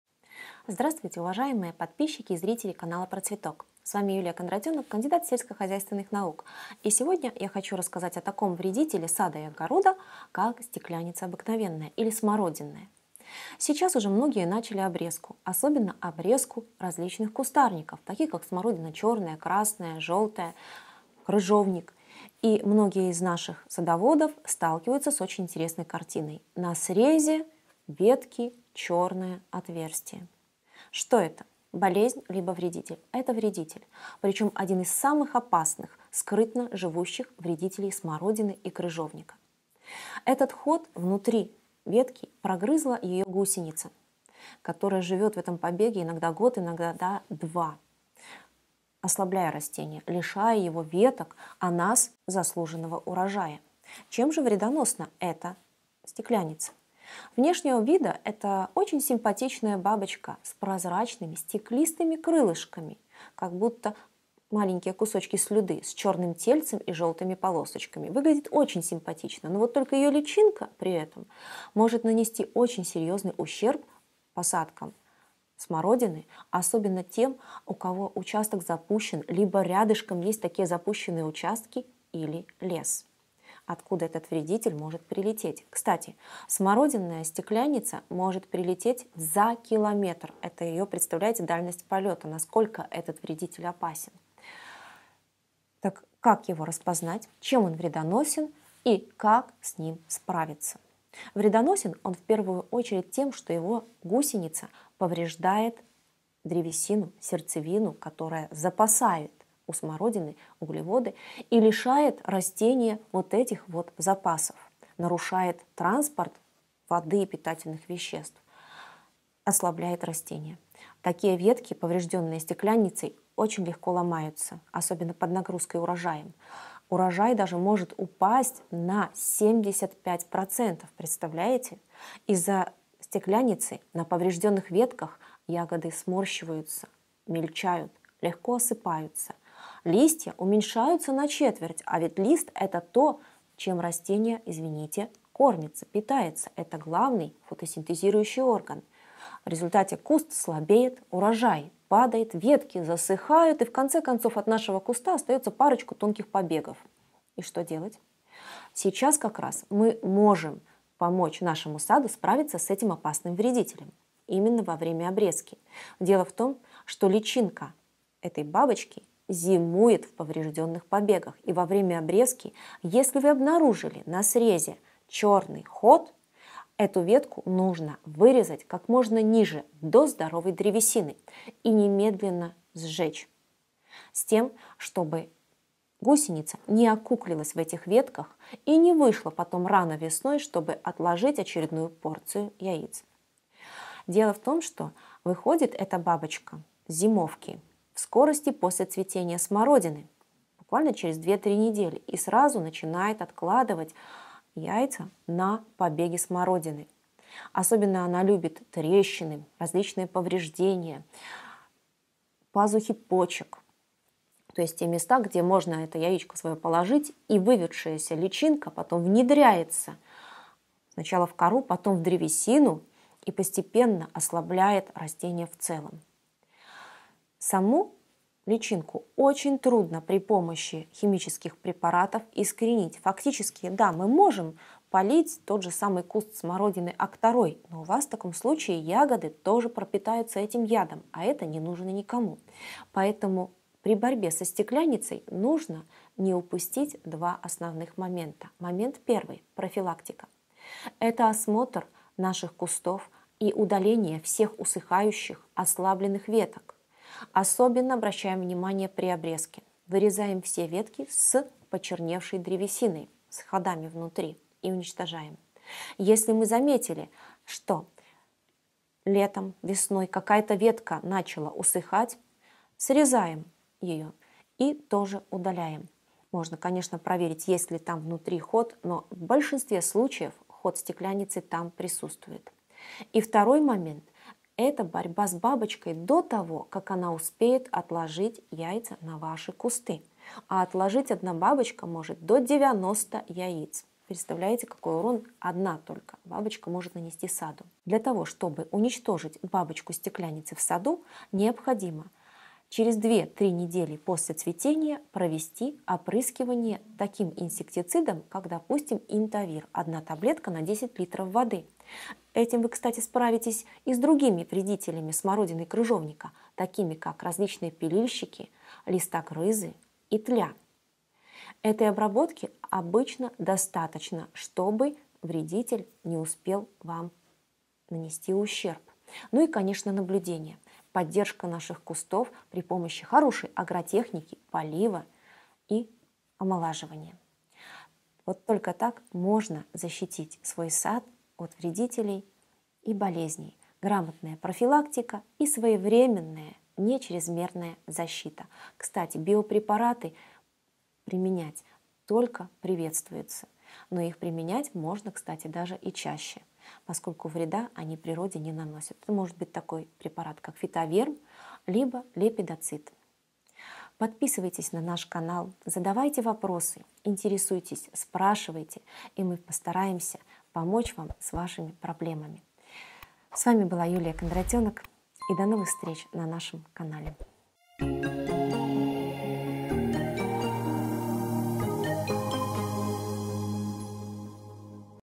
ВЫ ПРОСИЛИ РАССКАЗАТЬ – Как защитить сад от СТЕКЛЯННИЦЫ?
Опасный и скрытно живущий вредитель, смородинная стеклянница, способен лишить вас до 75% урожая ягод. А все потому что личинки этой бабочки живут внутри побегов, питаясь их сердцевиной, что значительно ослабляет растения, приводит к засыханию целых ветвей. Как защитить сад от стеклянницы? Какие профилактические меры предпринимать, чем можно обработать кусты смородины и когда? Вы просили рассказать об этом. И вот Юлия Кондратенок, селекционер-фитопатолог, кандидат сельскохозяйственных наук, разъясняет что нужно делать, чтобы стеклянницы на участке становилось все меньше, а в идеале она исчезла совсем.

00:00 Вступление
00:07 Стеклянница смородинная – распространенный вредитель
00:22 Признак поражения побега стеклянницей – черный на срезе
00:40 Как вредит личинка стеклянницы – прогрызает сердцевину побегов
00:54 Как выглядит и как вредит стеклянница – особенно ее гусеница
01:16 Что способствует распространению стеклянницы в посадках кустарников
01:41 Каким образом вредит гусеница стеклянницы, последствия для растения
02:38 Как обрезка помогает сократить популяцию стеклянницы
03:14 Когда стеклянница откладывает яйца на побегах смородины
03:47 Против личинок стеклянницы сложно подобрать препараты
04:09 1. Профилактическая обрезка – какие ветви удалять
04:25 Обрезку поврежденных веток продолжаем в течение всего сезона
04:43 2. Обработка против взрослых особей стеклянницы
04:57 Чем и когда провести обработку против стеклянницы
05:29 3. Правильная агротехника – для сопротивляемости растения вредителям
05:48 Биопрепараты против стеклянницы на смородине – особенности применения
06:03 Заключение